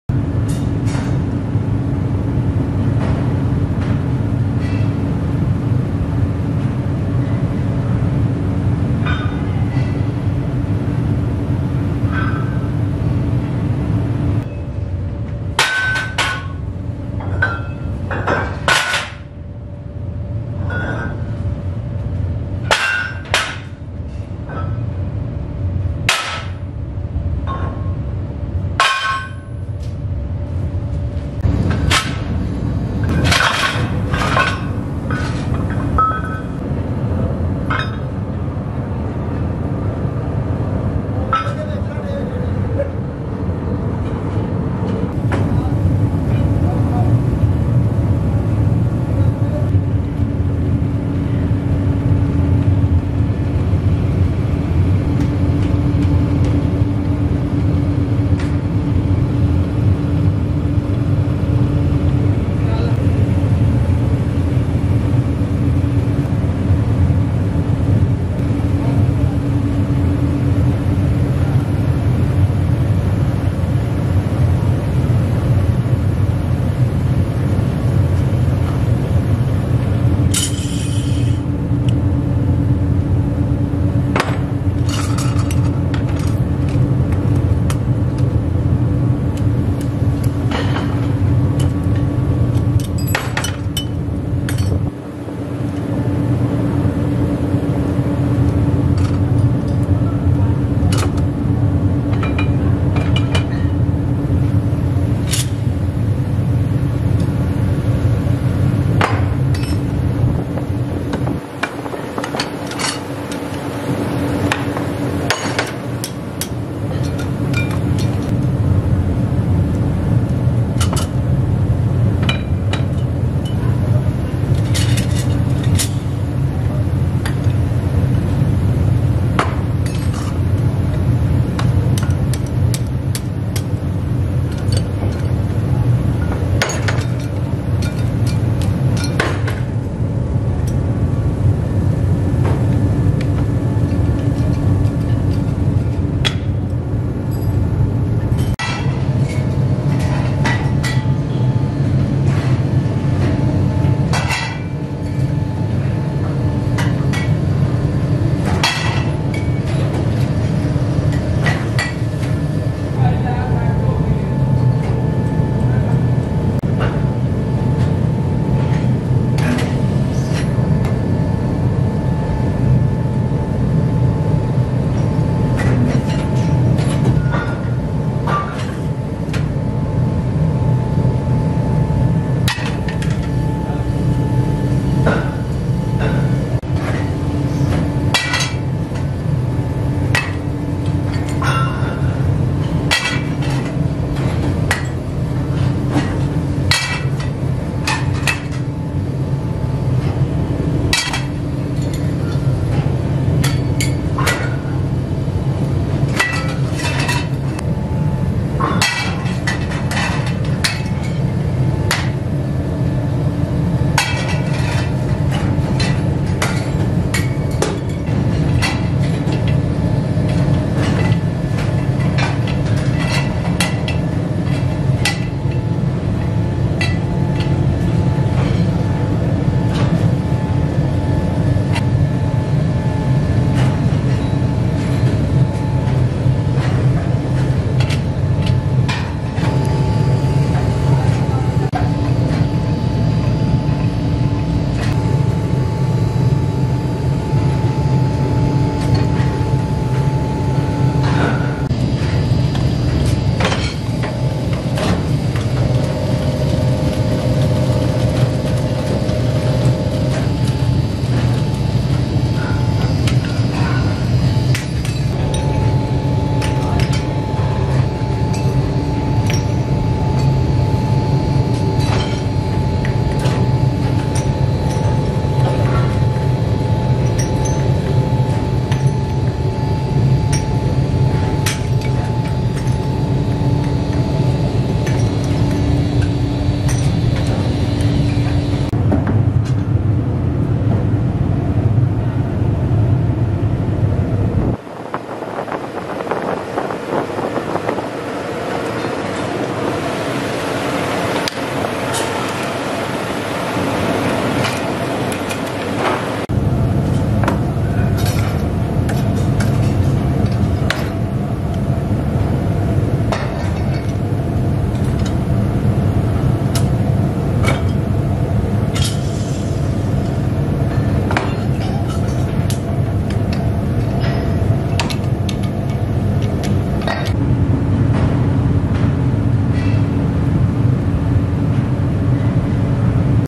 Watch How This Incredible Factory Transforms Aluminum Into Ceiling Fans Parts
Looking for a workplace that transforms aluminum into ceiling fans parts? Check out this incredible factory! In this video, you'll get a tour of the factory and see how they transform aluminum into ceiling fan parts.

This factory is a great place to work if you're looking for a challenging and exciting job. The workers here are required to use both manual and automated machines to produce ceiling fan parts. This is a fascinating factory that will show you how aluminum is transformed into the products you love!